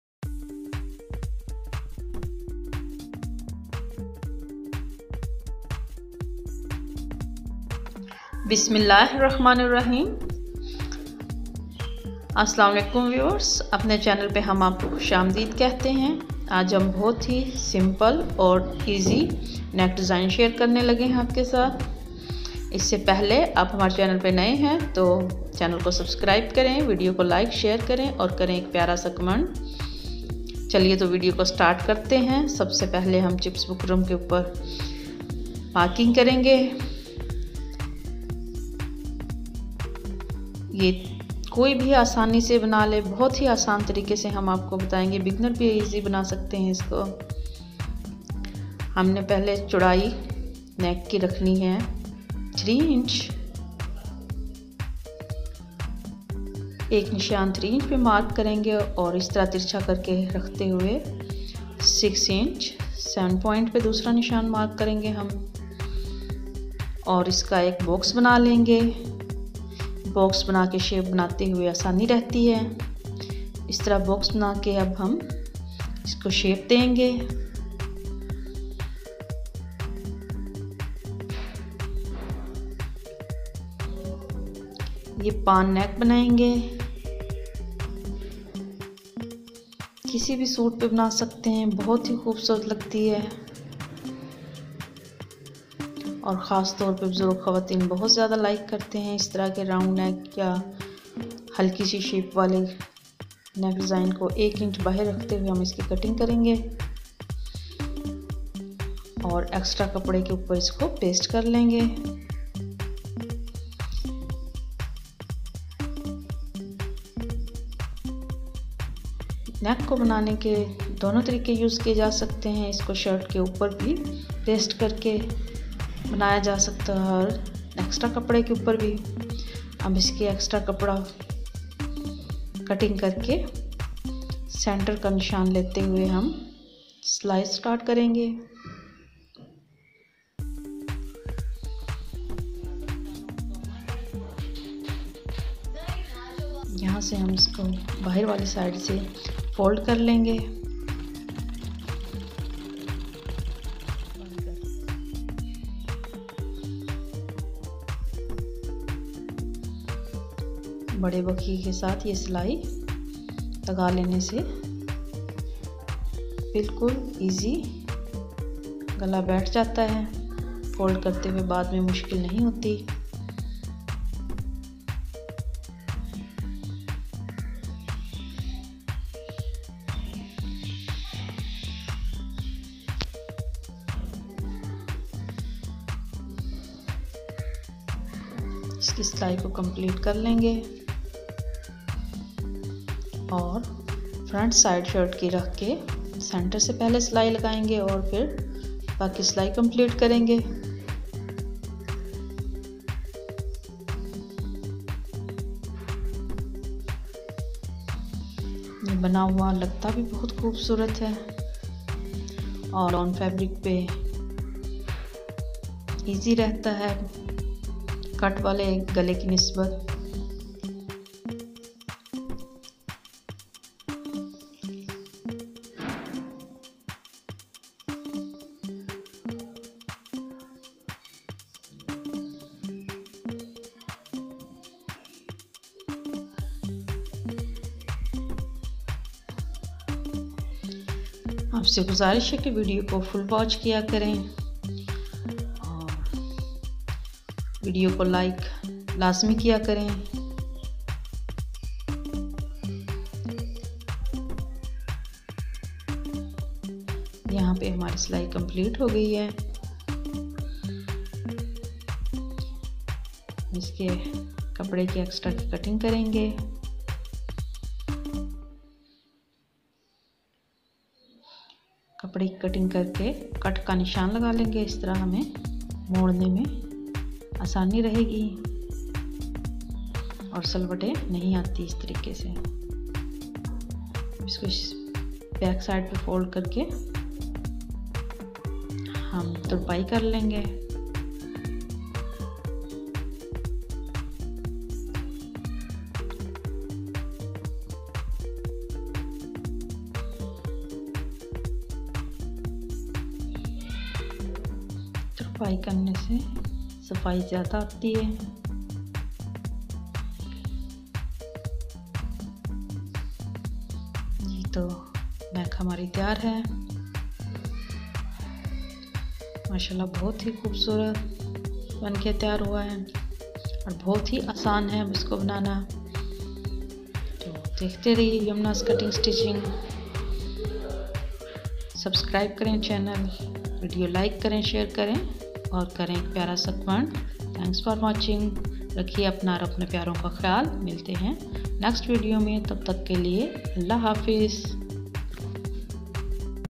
Pan neck 💕💕💕# Yamna,s cutting stitching
Pan neck bnane ka treeka sher kare ge beautiful neck bnane ka . assn treeka batten ge bigner bhi easyli bna sakte hn mack pan neck in easy method . easy cutting stitching . stau with us Yamuna stitching cutting# Yamuna cutting stitching